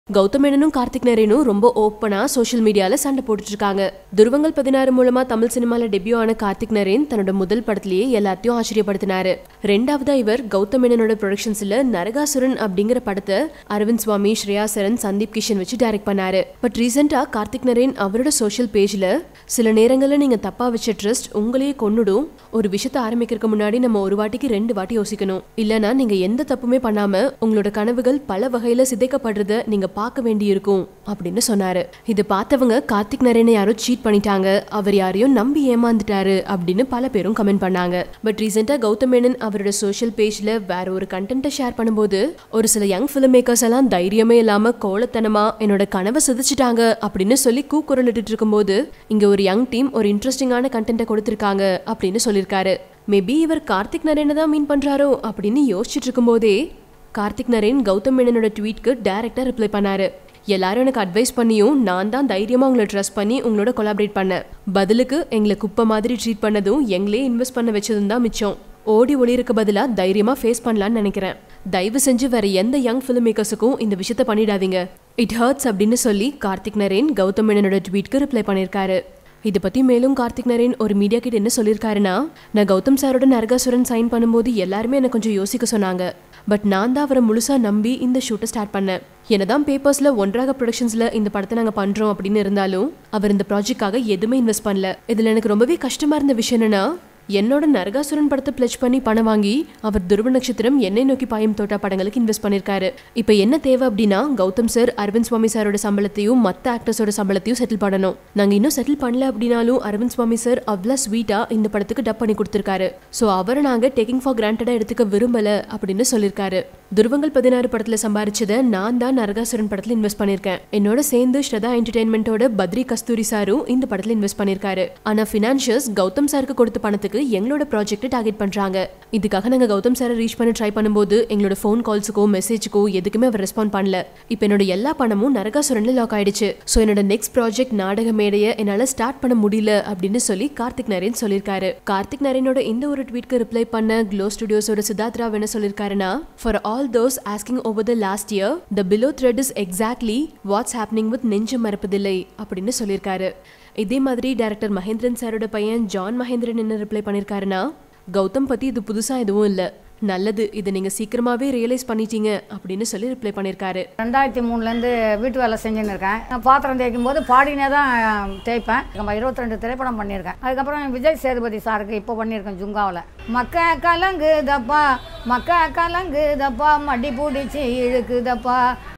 Open Fight between Director Gautham Menon & Karthik Naren | Latest Tamil Cinema News
Gautham Menon agreed to produce Karthik Naren's Naragasooran. Yesterday, they both had a clash over Twitter where Karthik Naren mentioned that he has been cheated without mentioning the person's name.